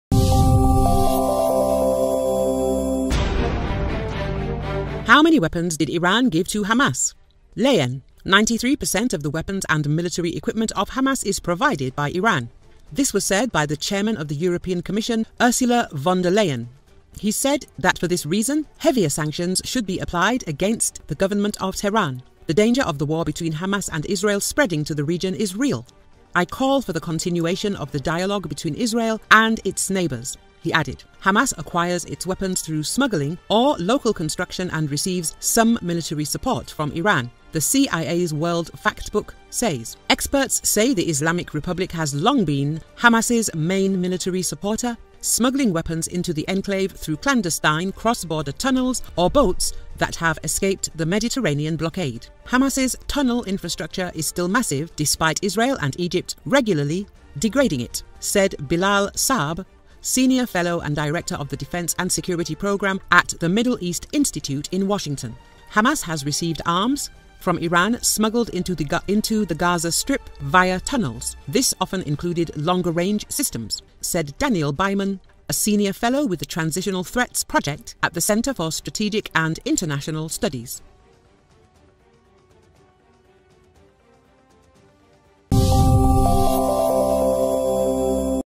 Iran is the largest supplier of weapons to Hamas - Head of European Commission
Some 93 per cent of the weapons and military equipment of Hamas is provided by Iran. This was said by the chairman of the European Commission, Ursula von der Leyen.
He said that for this reason, heavier sanctions should be applied against the government of Tehran.
"The danger of the war between Hamas and Israel spreading to the region is real. I call for the continuation of the dialogue between Israel and its neighbours," he added.
“Hamas acquires its weapons through smuggling or local construction and receives some military support from Iran,” the CIA’s World Factbook says.
Experts say the Islamic Republic has long been Hamas’ main military supporter, smuggling weapons into the enclave through clandestine cross-border tunnels or boats that have escaped the Mediterranean blockade.
“Hamas’ tunnel infrastructure is still massive despite Israel and Egypt regularly degrading it,” said Bilal Saab, senior fellow and director of the Defense and Security Program at the Middle East Institute in Washington.
“Hamas has received arms from Iran smuggled into the Gaza Strip via tunnels. This often included longer-range systems,” said Daniel Byman, a senior fellow with the Transnational Threats Project at the Center for Strategic and International Studies.